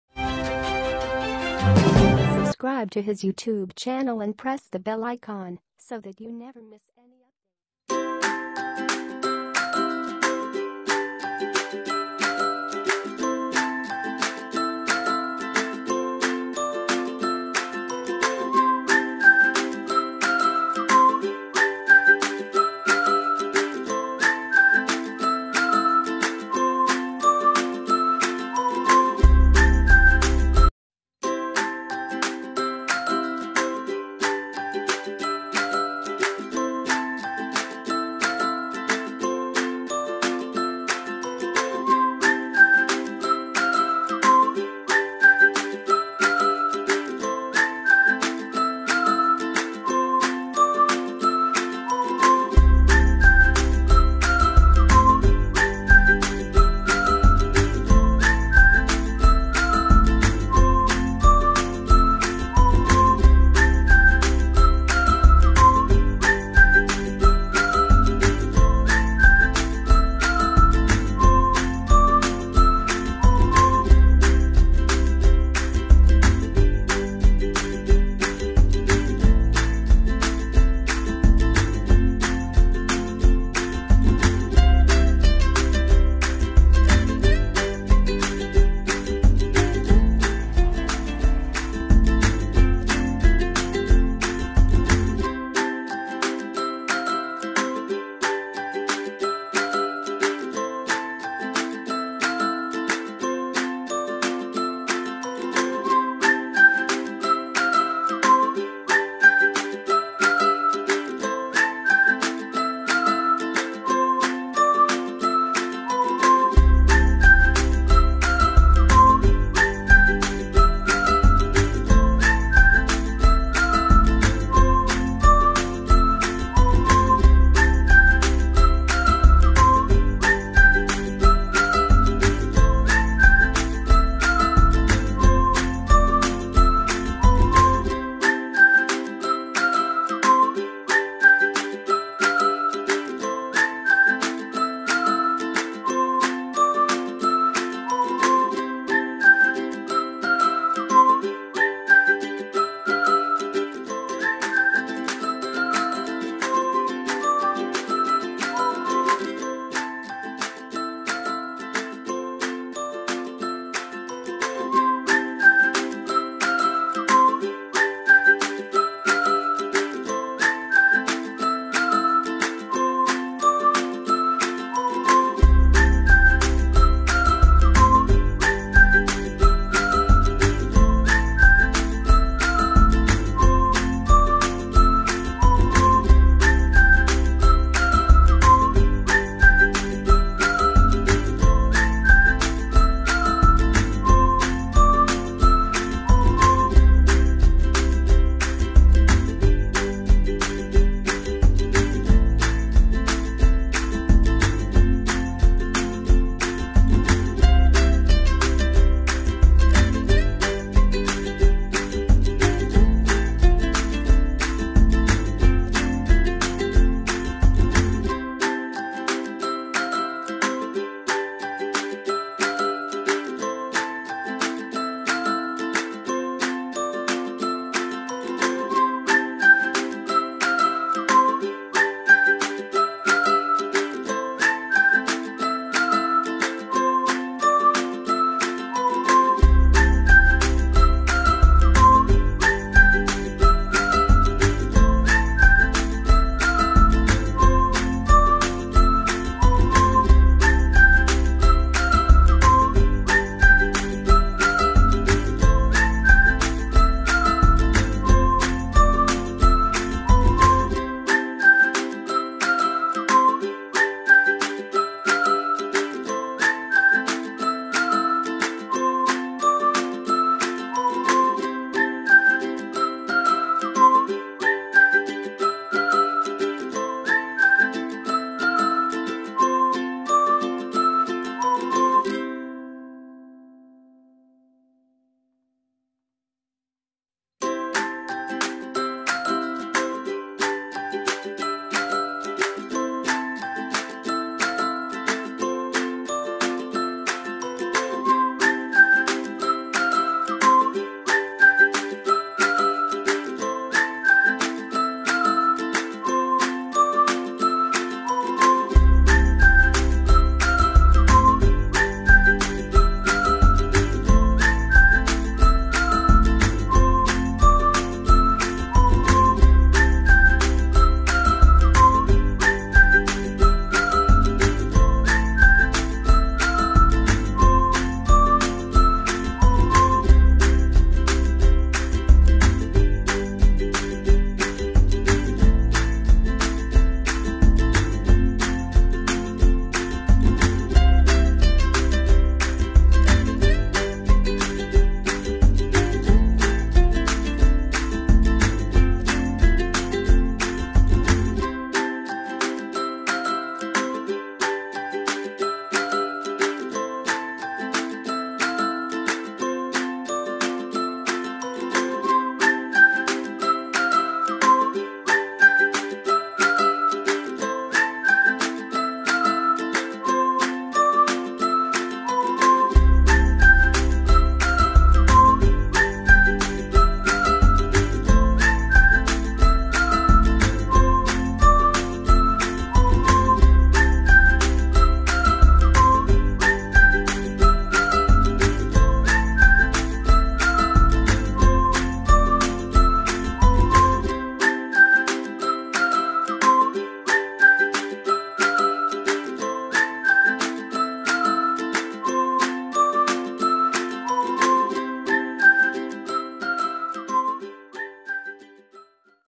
DIY/आइसक्रीम की स्टिक से बनाएं सुन्दर MICKEY MOUSE KEY HOLDER At Home New Idea /Craft Stitching
hello friends
mickey mouse key holder mkaing